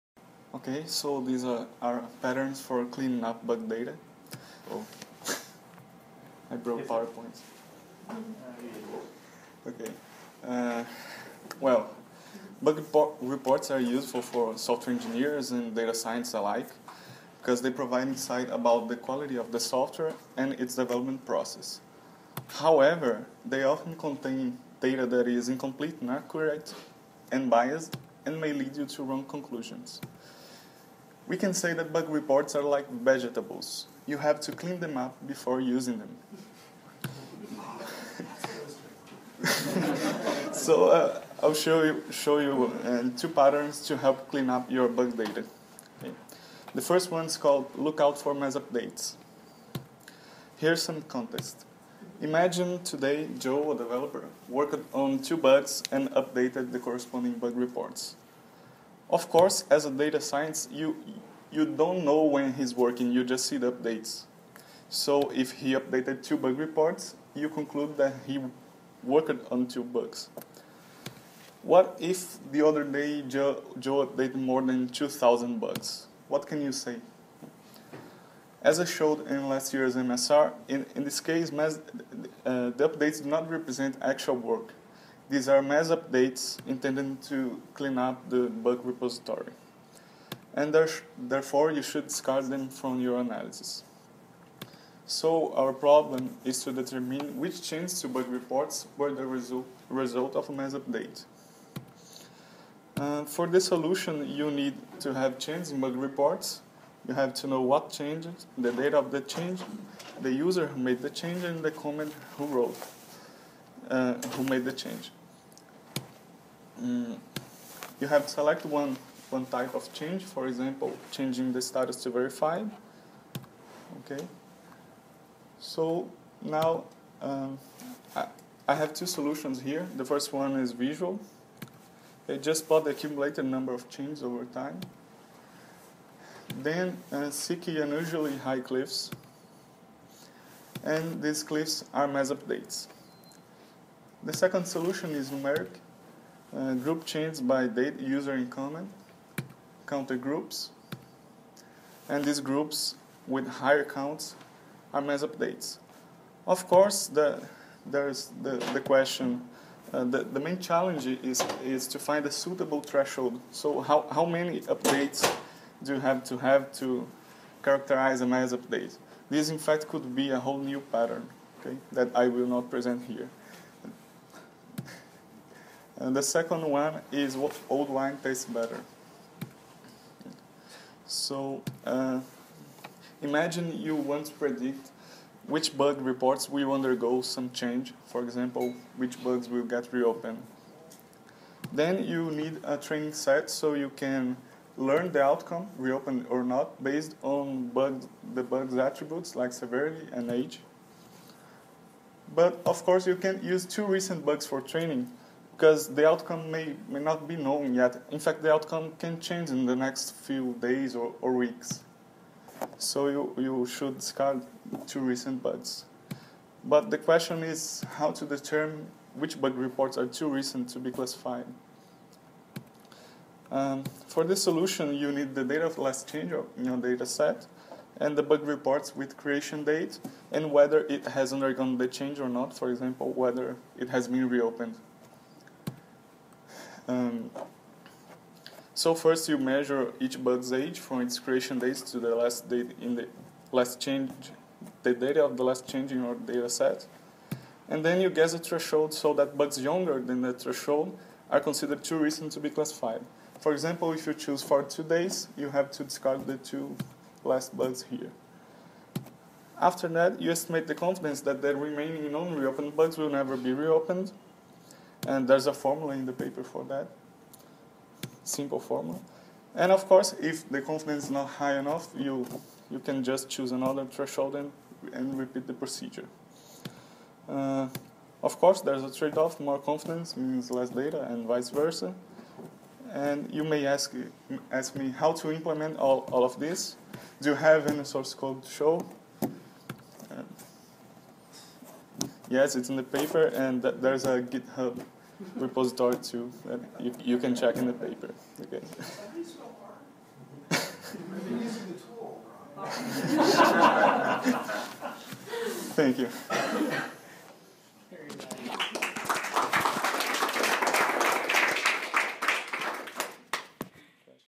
Patterns for Cleaning Up Bug Data
Talk at the 1st International Workshop on Data Analysis Patterns in Software Engineering (DAPSE 2013), collocated with ICSE 2013, in San Francisco, CA, USA.